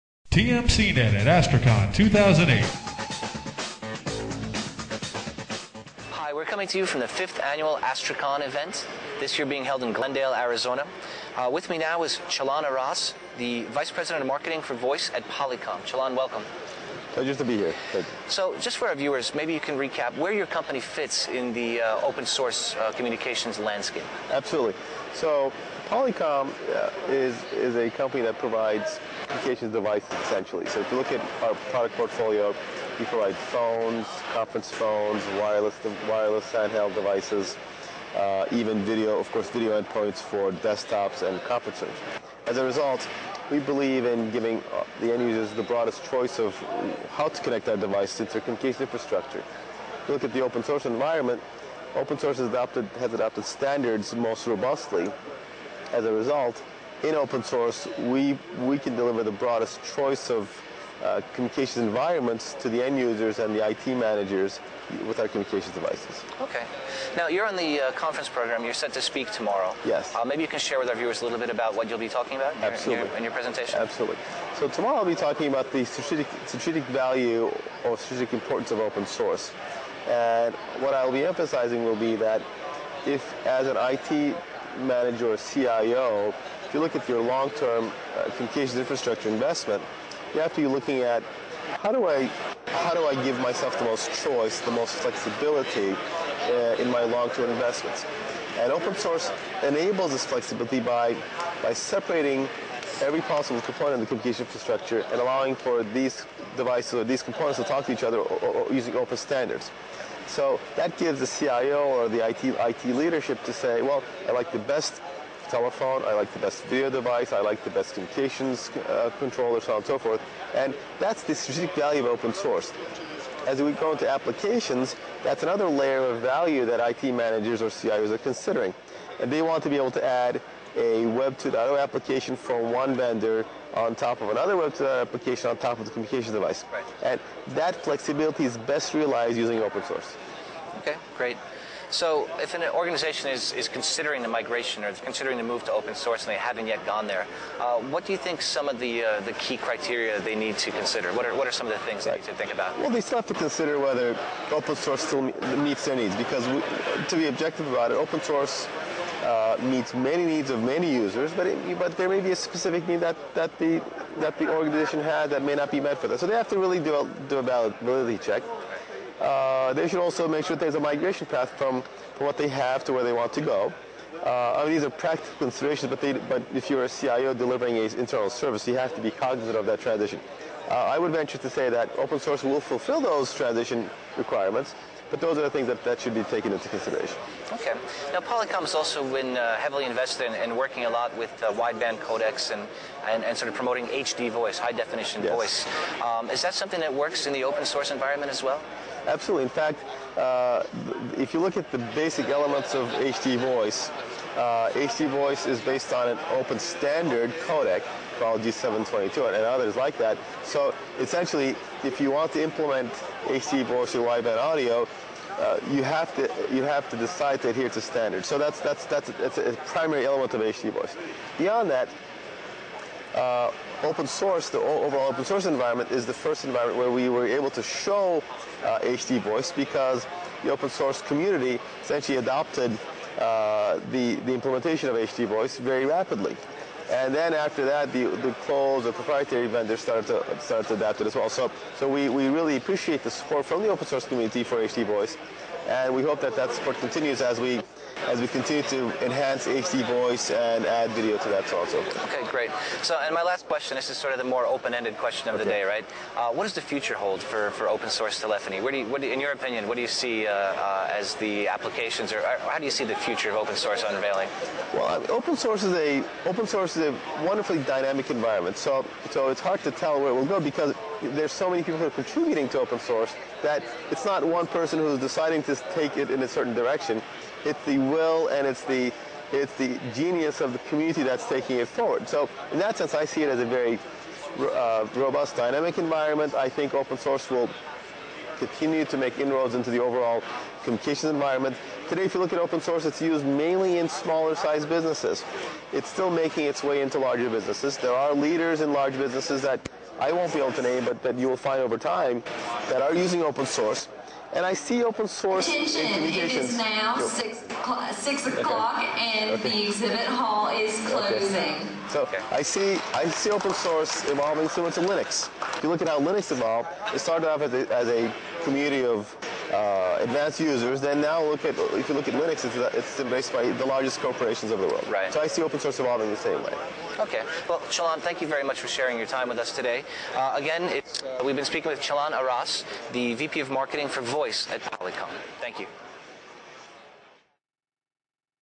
Polycom Interview
Interview with Polycom VP of Marketing, Chalan Aras - Oct 02, 2008